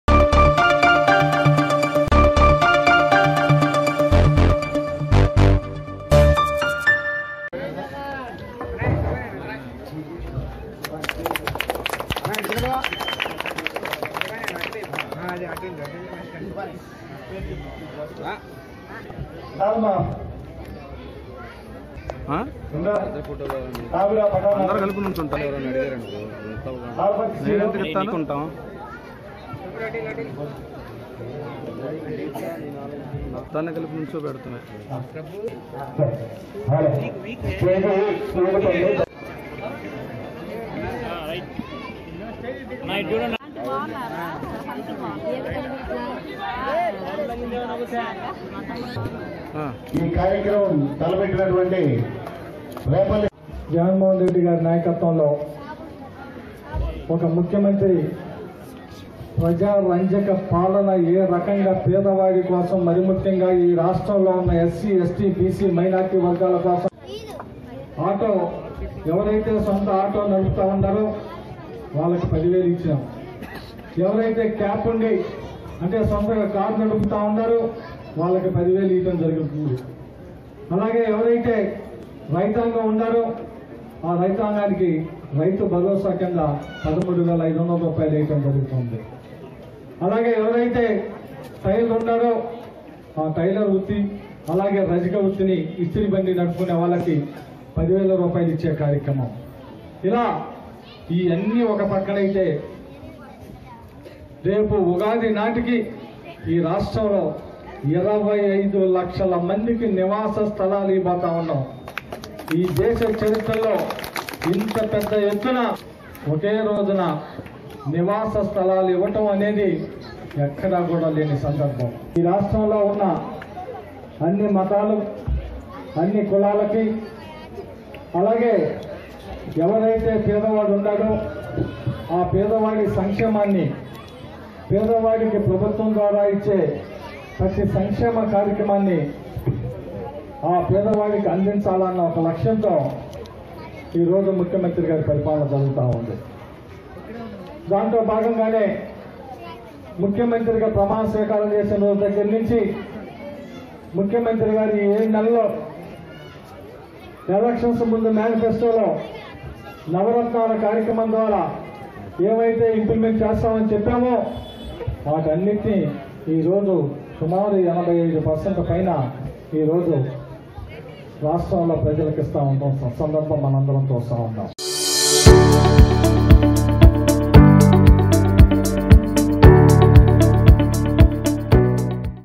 New Joinings In Presence Of YSRCP MLA Annabattuni Sivakumar | Tenali | AP CM YS Jagan | ALO TV
New Joinings In Presence Of YSRCP MLA Annabattuni Sivakumar | Tenali | AP CM YS Jagan |